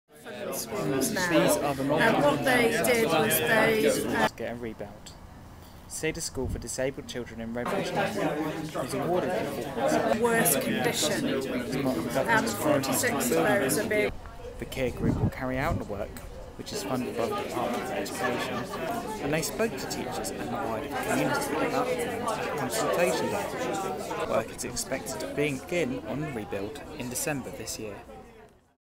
Cedar school rebuild plans
Cedar School in Redbridge Lane, Southampton is set to have a £4,7 million pound rebuild.